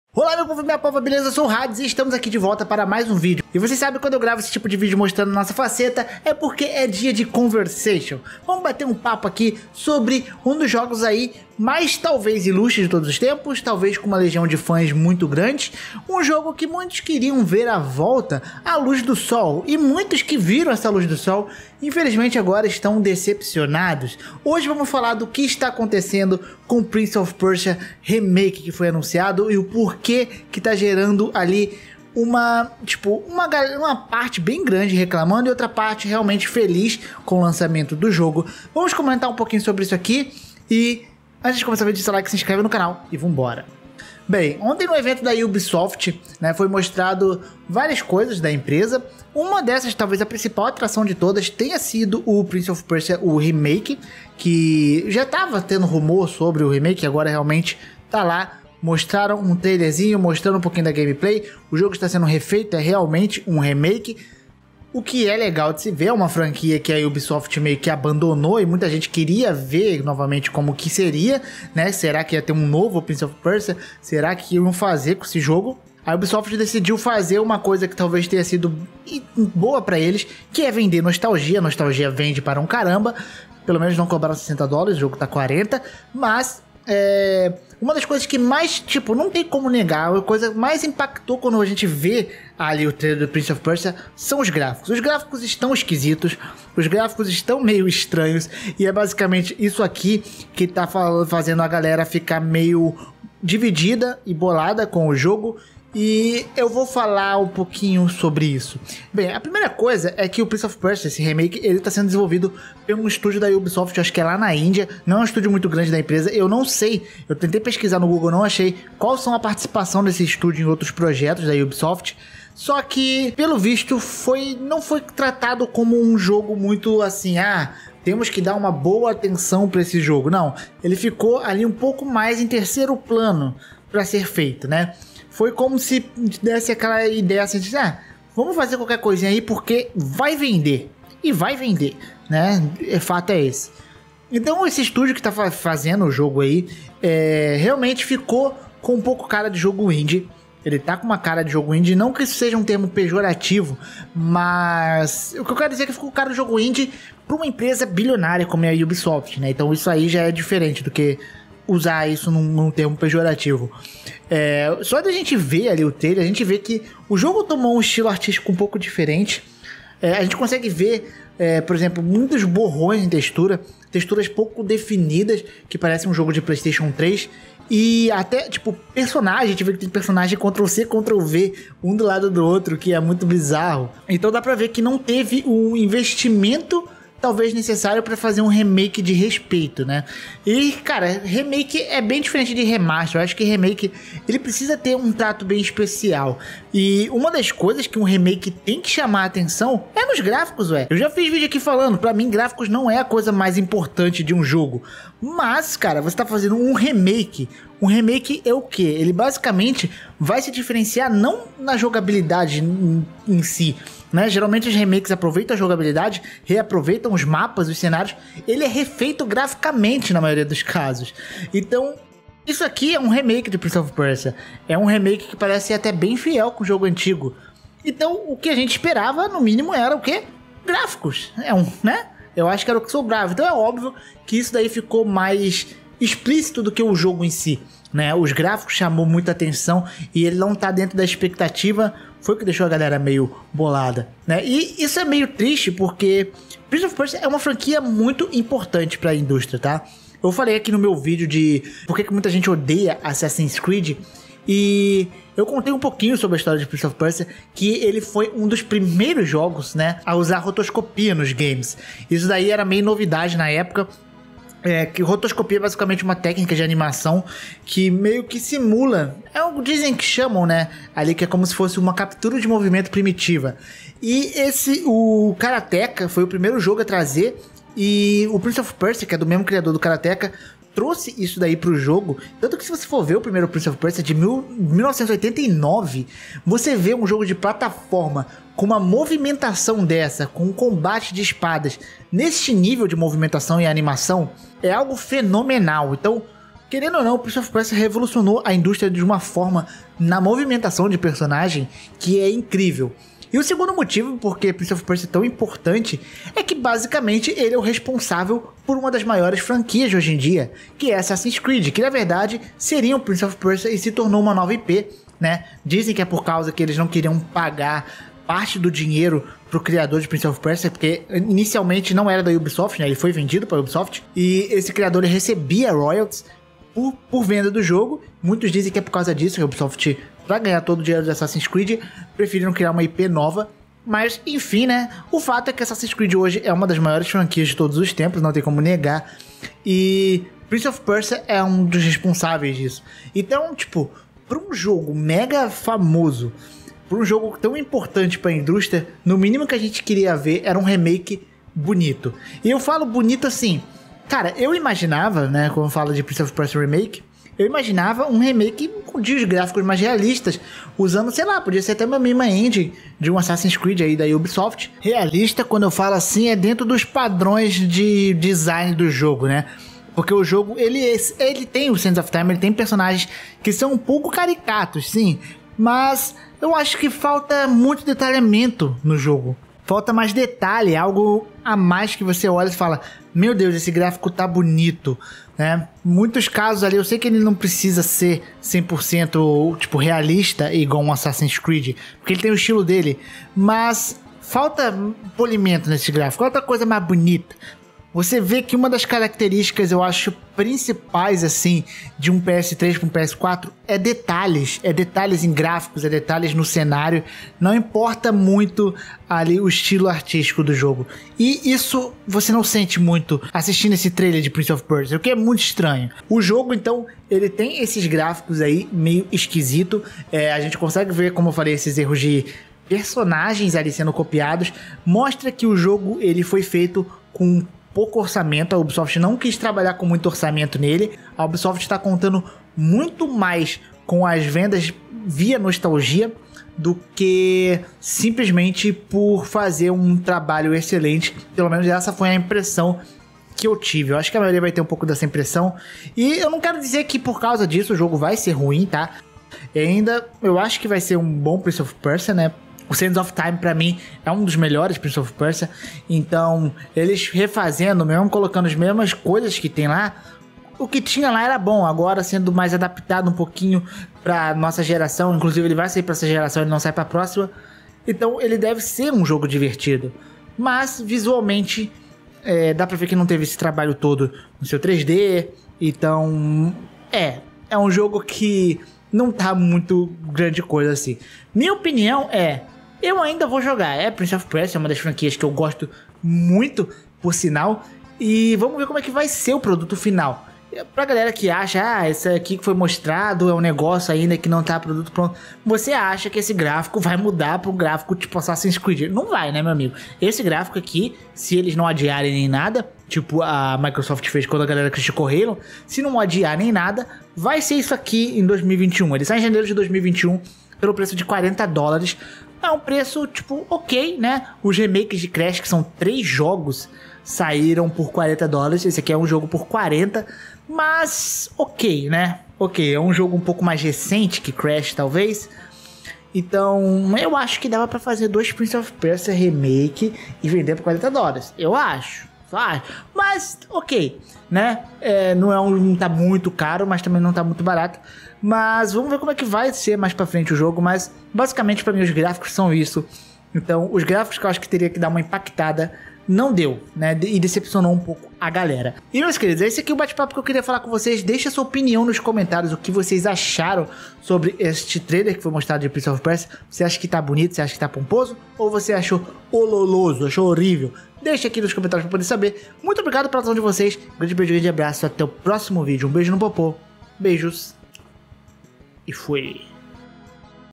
Prince of Persia remake ta tão HORRÍVEL assim?
Novo remake de Prince of Persia sands of time anunciado e já deixou uma boa parte da galera meio chateada.

➤ PSN: hadessama94